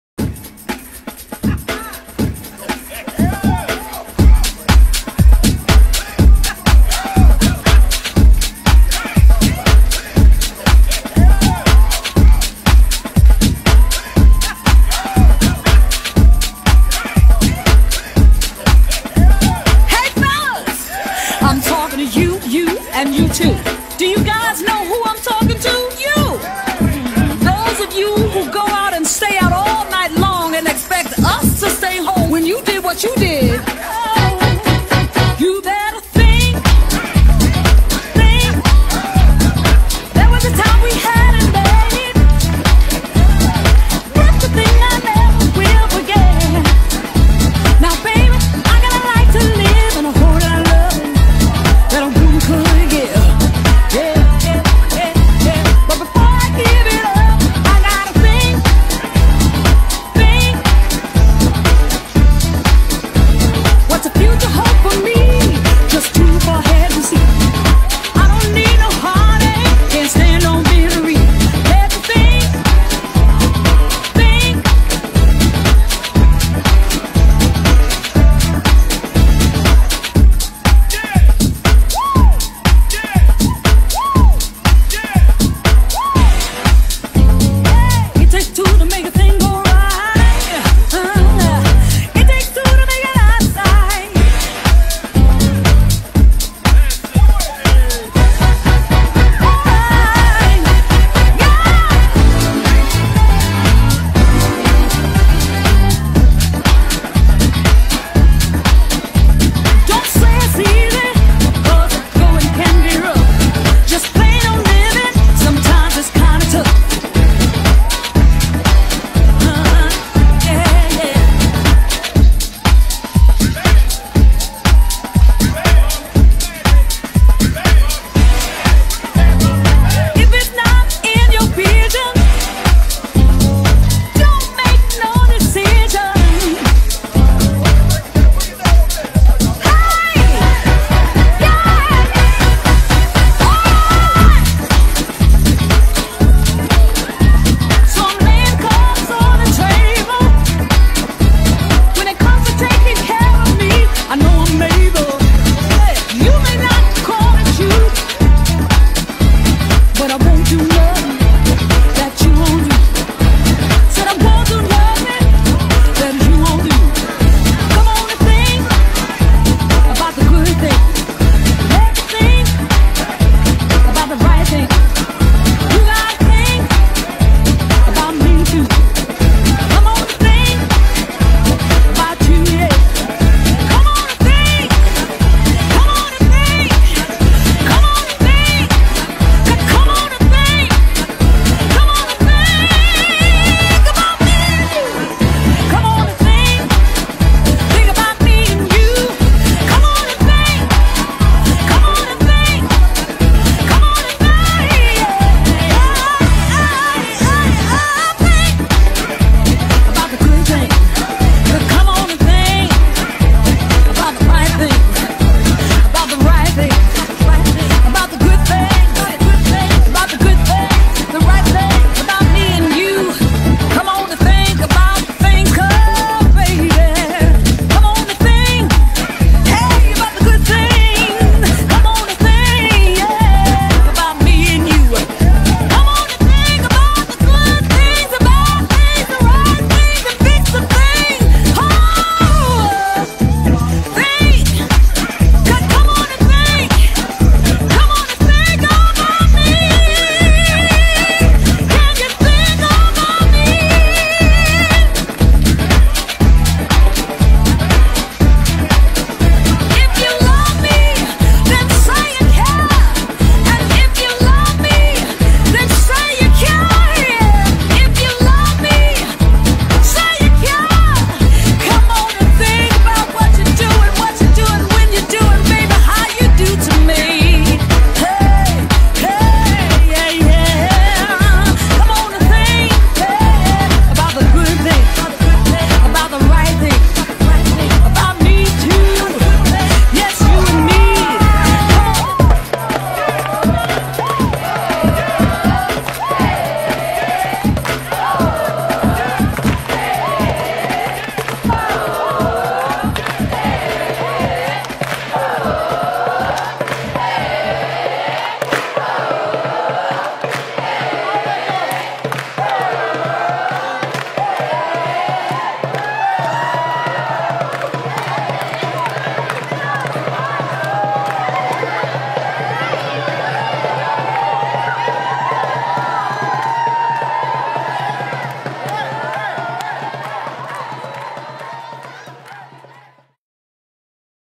Le SoulTrain de fin 2017 par Eric Koloko
Le cadeau de Noël avant Noël : un concentré de bonne humeur pour ce Soul Train 100% efficace !
Merci encore à toutes et à tous pour votre investissement dans ce projet intergénérationnel ! A partager sans modération :)
Merci également à la LMJSM Gymnastique pour la salle :) et VP Motion pour la réalisation vidéo !